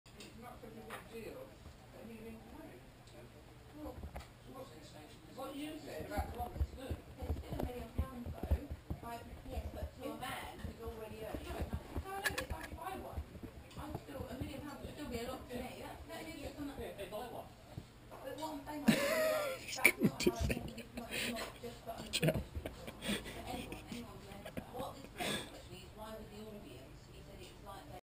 Cute kitten getting dizzy
Our crazy new kitten chasing the lazer pen round and round and round.

Filmed on a Go Pro Hero HD 720p @ 60fps has been compressed and cut.